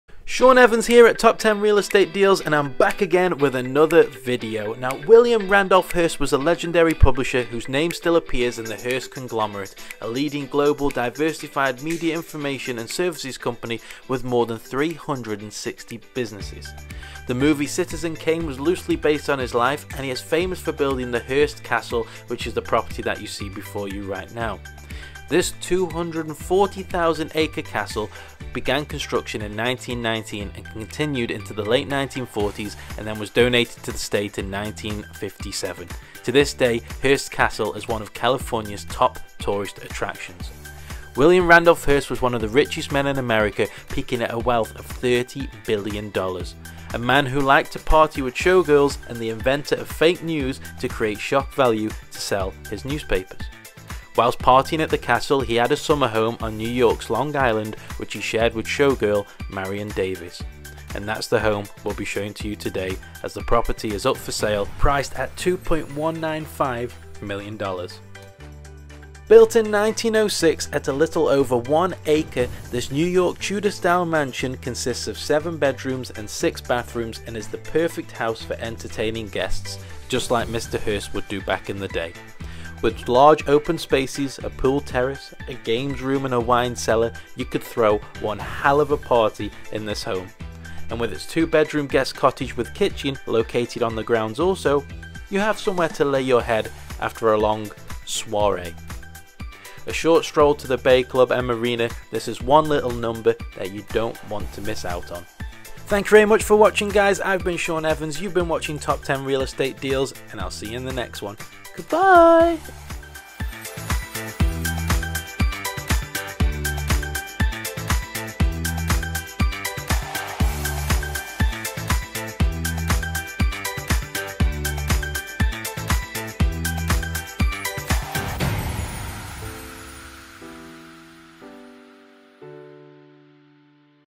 Inside William Randolph Hearst's $2.195 Million Mansion
Built in 1906 on a little over one acre, the New York Tudor-style mansion was one of five built near the original Beaux Arts Casino, now known as the Bay Club.

This seven bedroom, six bathroom property is currently on the market for $2.195 million.

Hearst Castle images under license from Shutterstock.com
NY Propery Photo Credit - Douglas Elliman